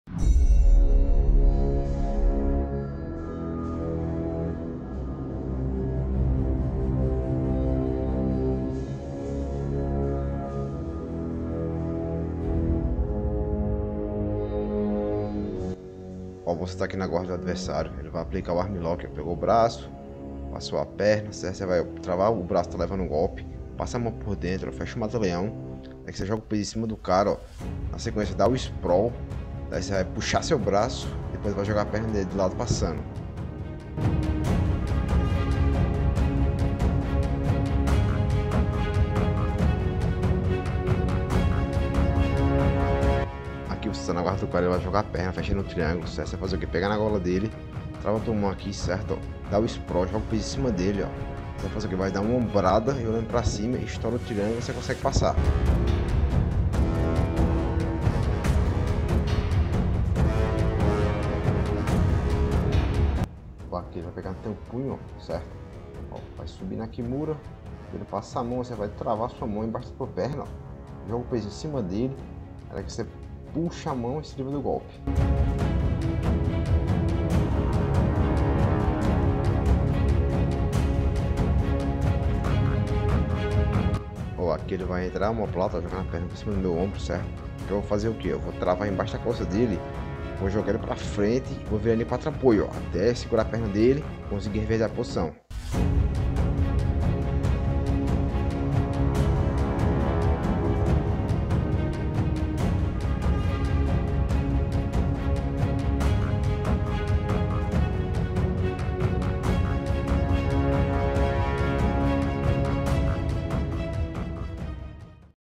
SAÍDAS DE GOLPES BASICOS DO JIU-JITSU - [PASSO A PASSO]
No video eu mostro algumas saídas de golpes básicos do jiu-jitsu, que vão te ajudar muito na hora do seu treino ou competição!!